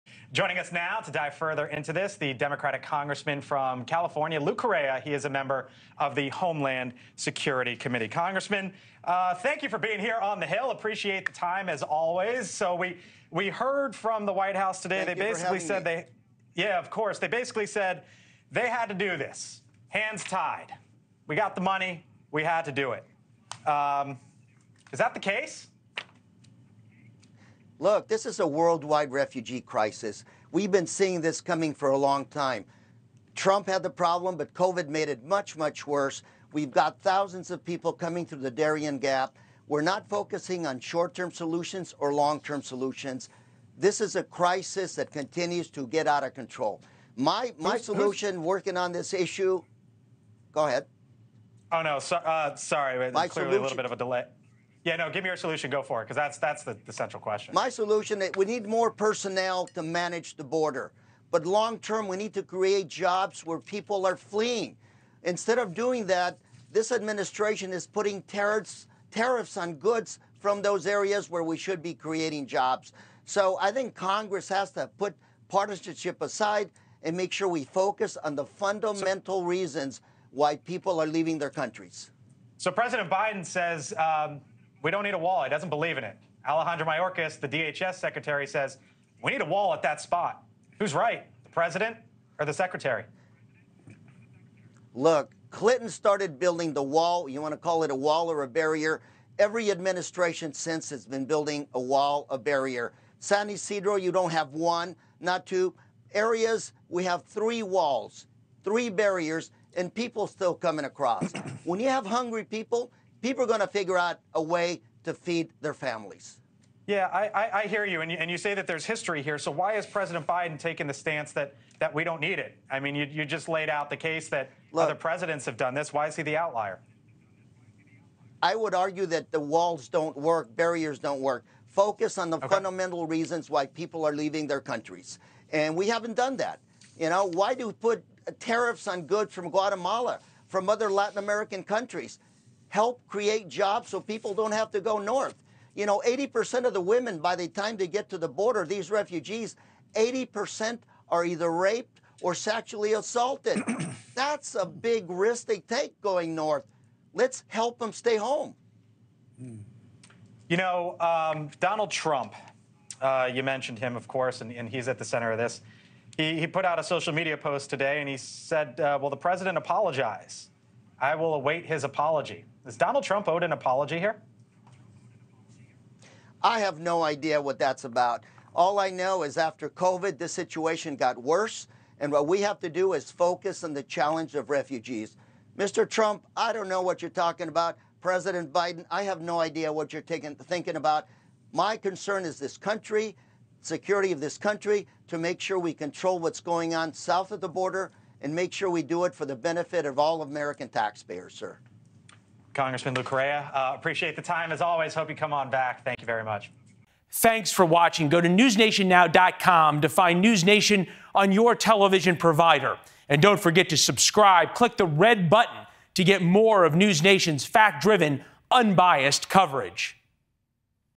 Democratic congressman: "Walls don't work, barriers don't work" | The Hill
The Biden administration announced it would waive some federal regulations to build part of a border wall. California Congressman Lou Correa argued walls won't solve America's immigration problems.